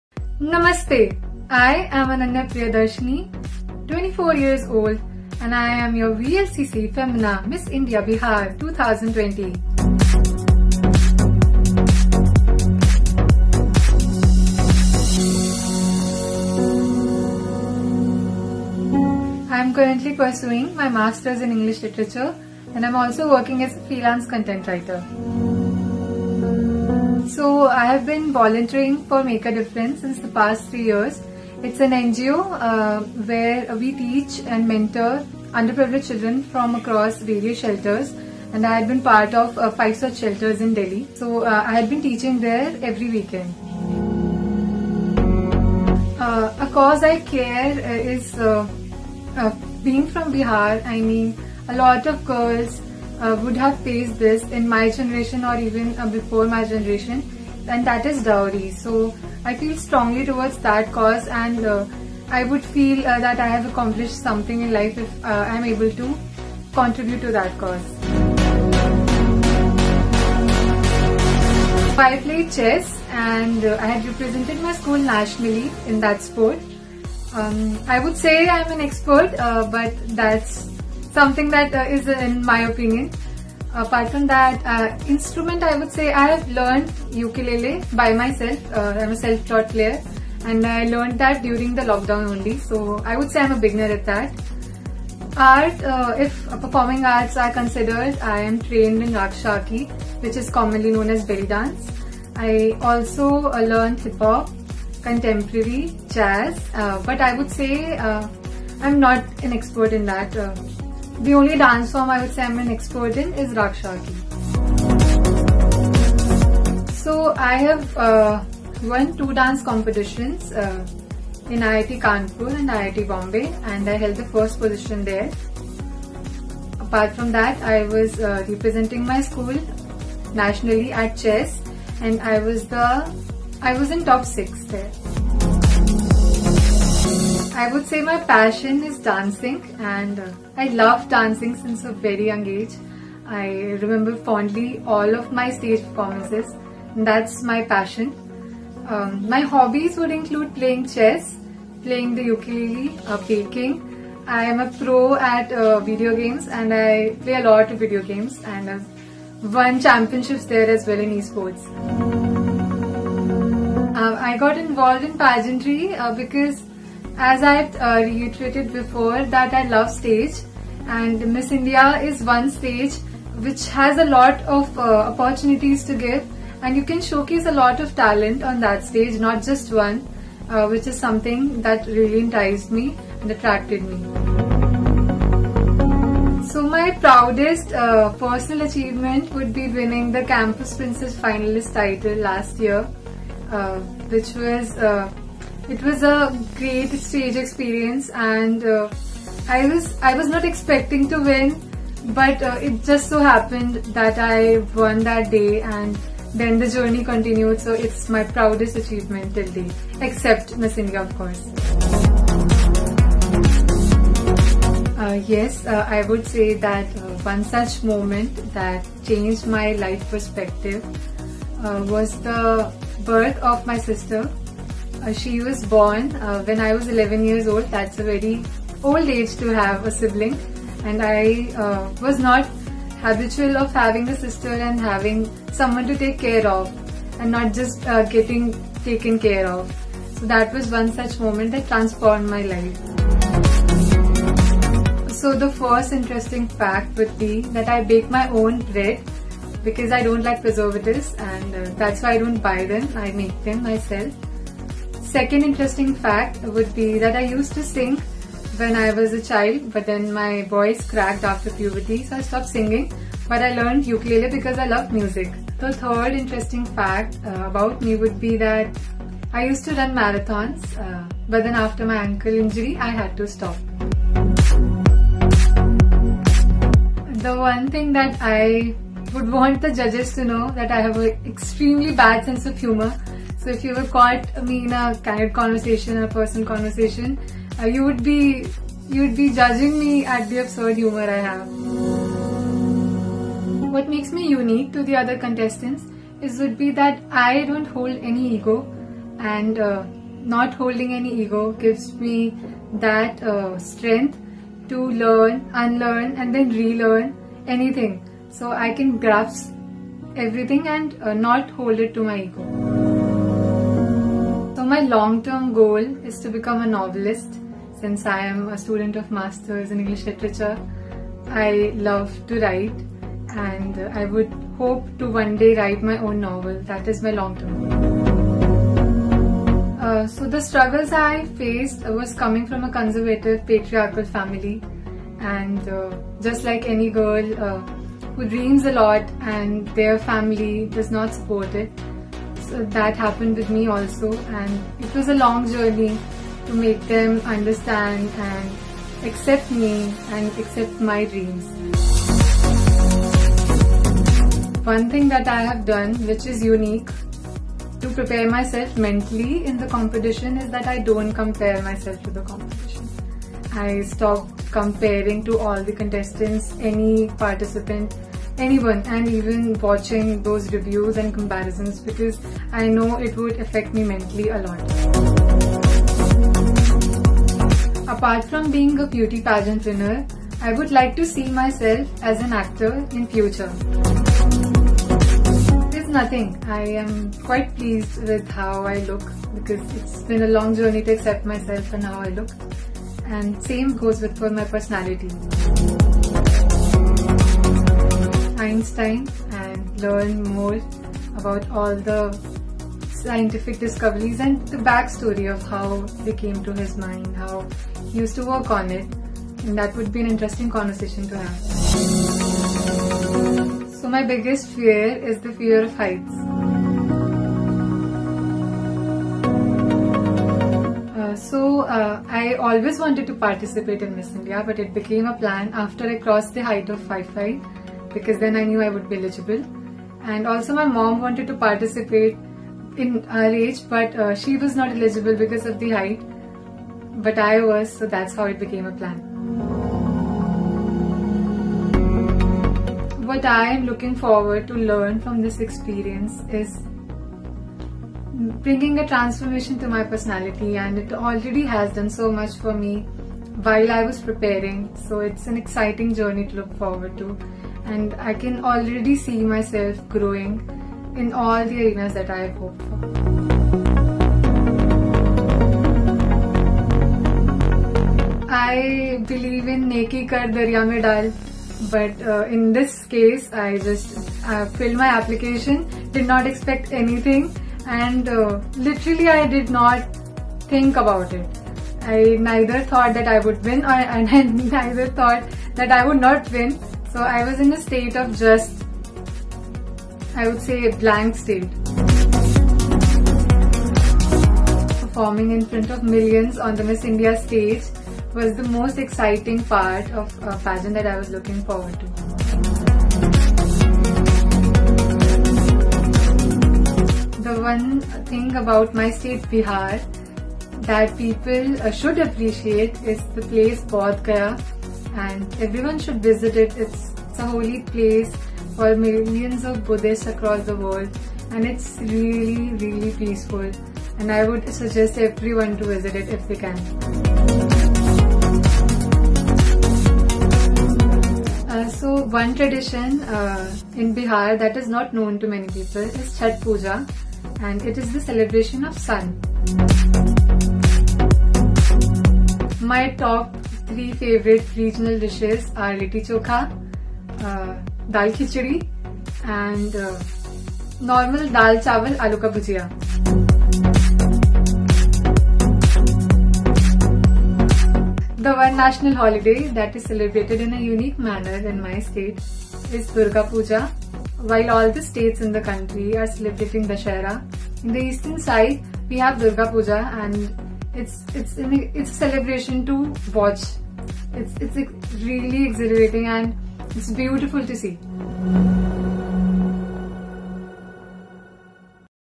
Ananya Priyadarshini, VLCC Femina Miss India Bihar 2020, opens up to us about her hobbies...
Ananya Priyadarshini, VLCC Femina Miss India Bihar 2020, opens up to us about her hobbies, passions, awards and more. @ananyapriyadarshinii

Exclusive Broadcast Partner: @colorstv HD